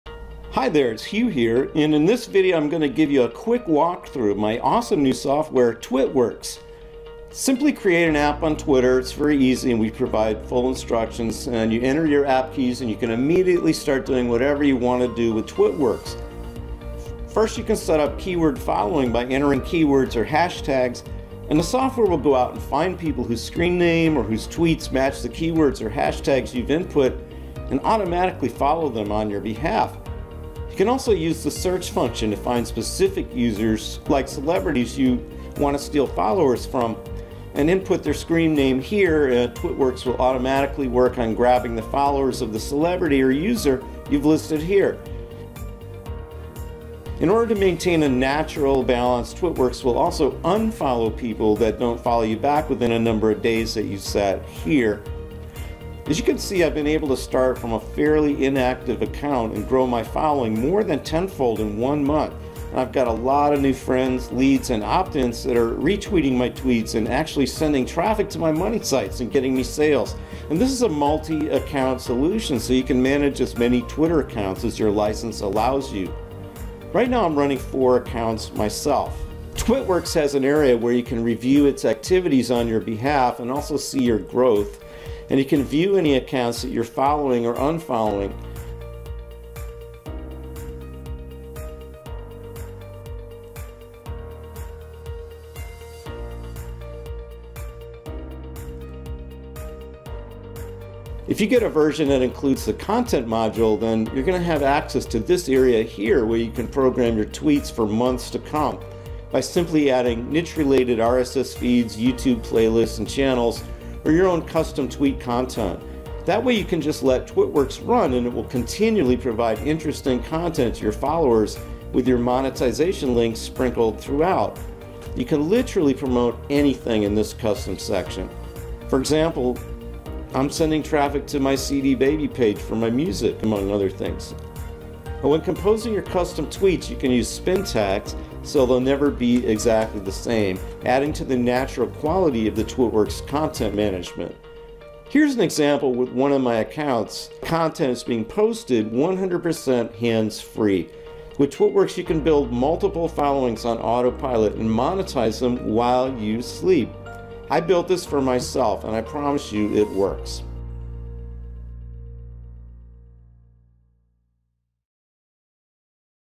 Twitworkz Software Walkthrough
In this video Hugh walks you through all of the main functions of the new Twitworkz software.